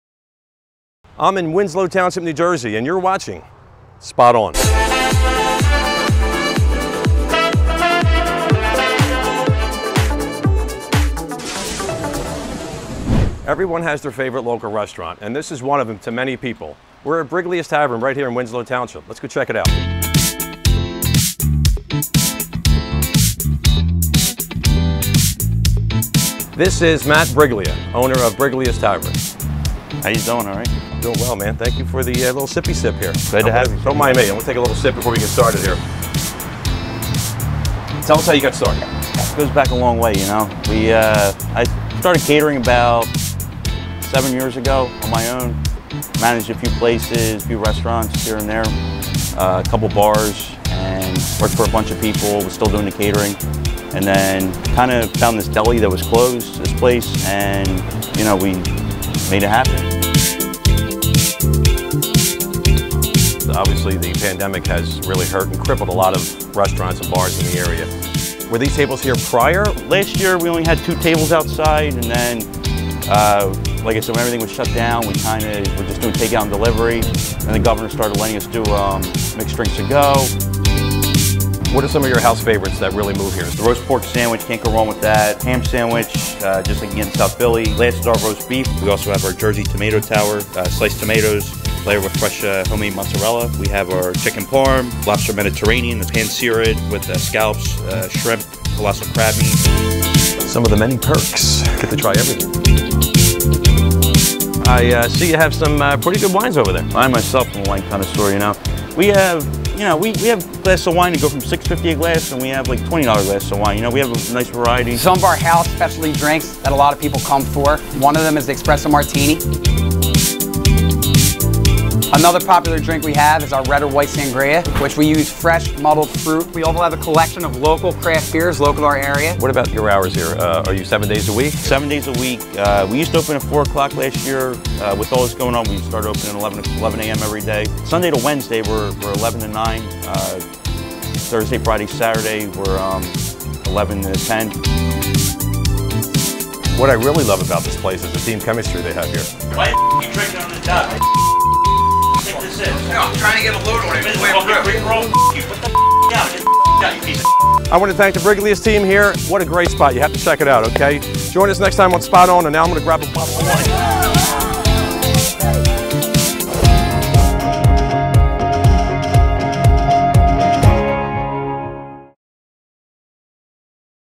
Briglia's Tavern in Winslow Township, NJ. (Spot On episode #5)
This week we are featuring Matt Briglia of Briglia's Tavern in Winslow Township, NJ. This place has it all from your traditional bar food to great entrees. Great cocktail and wine selection too!